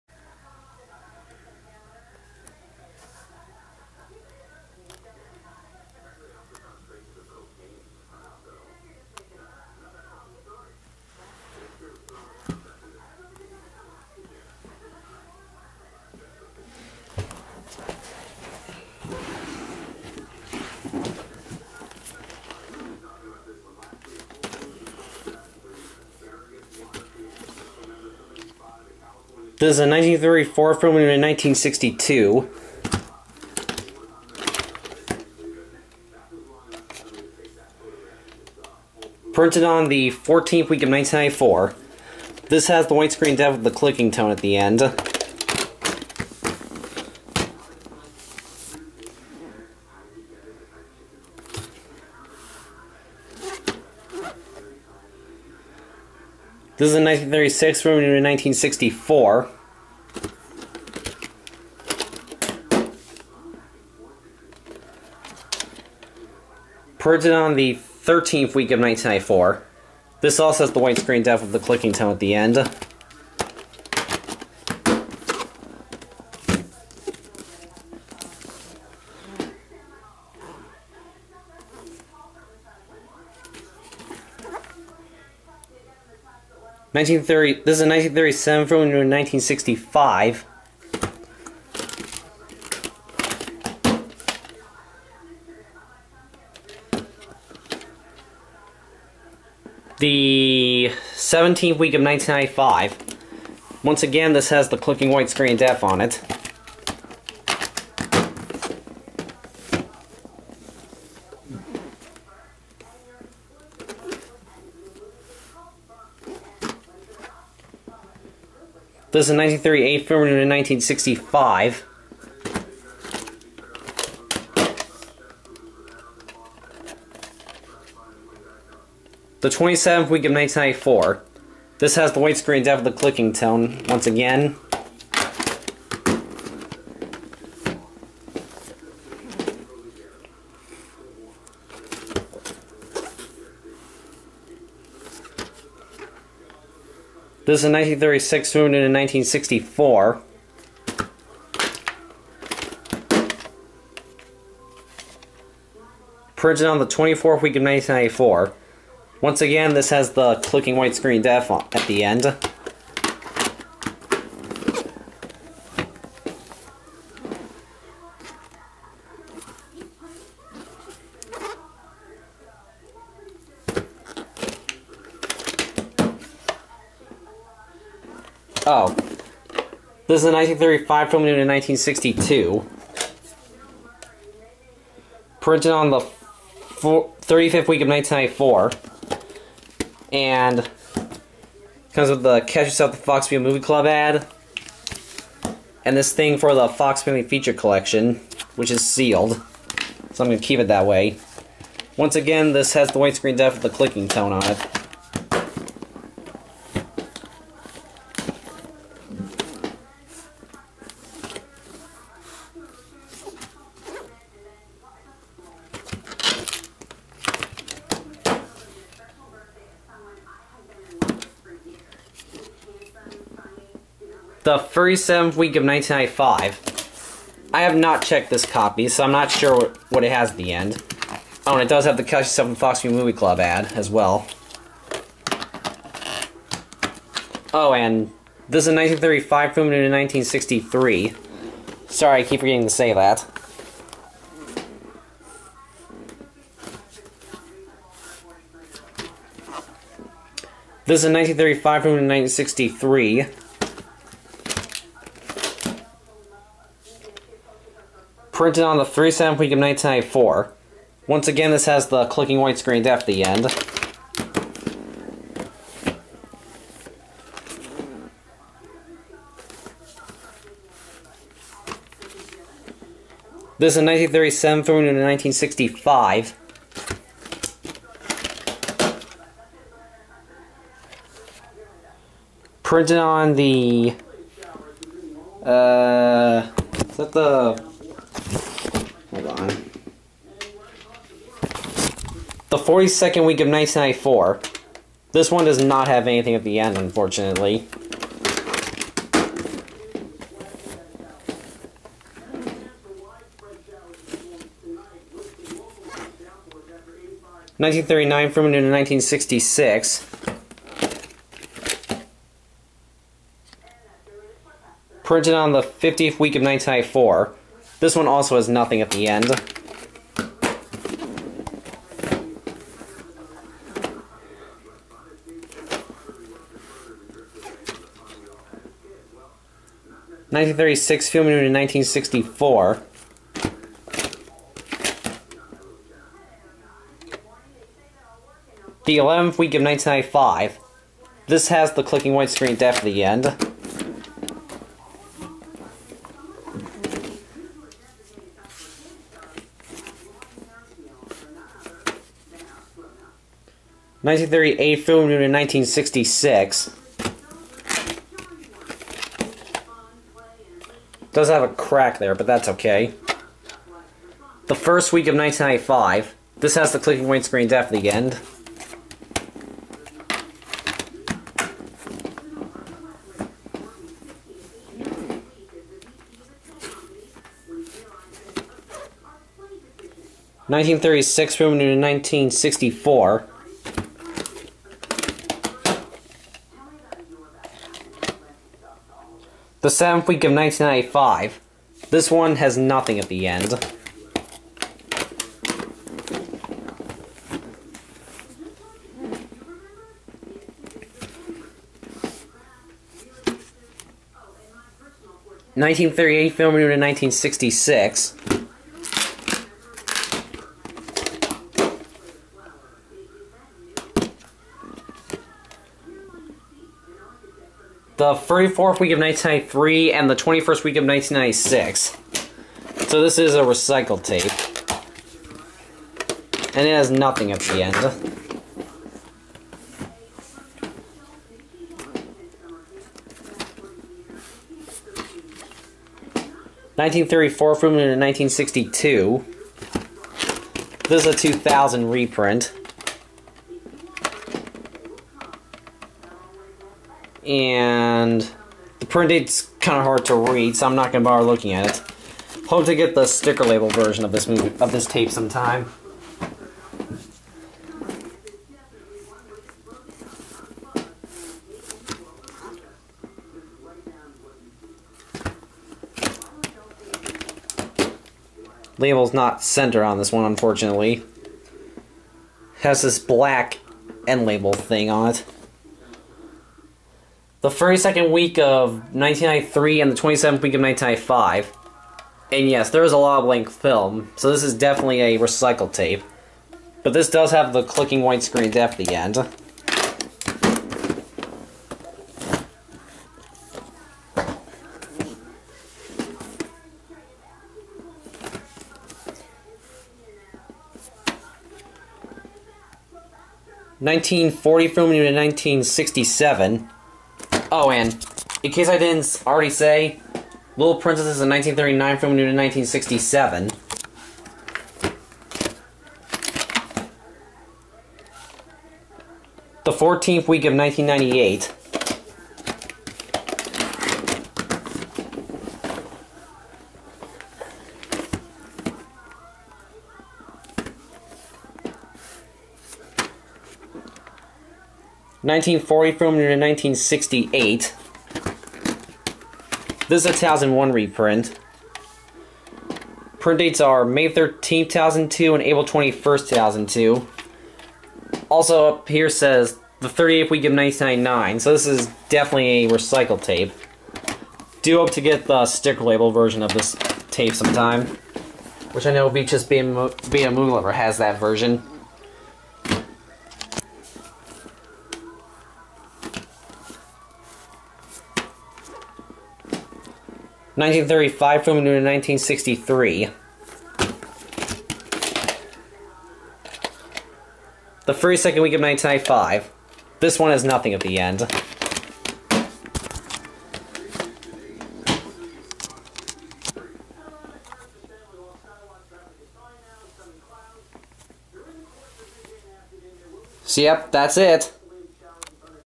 All 19 Volumes In The Shirley Temple Family Feature VHS Collection!
One Of My Favorite Collections Within My Collection.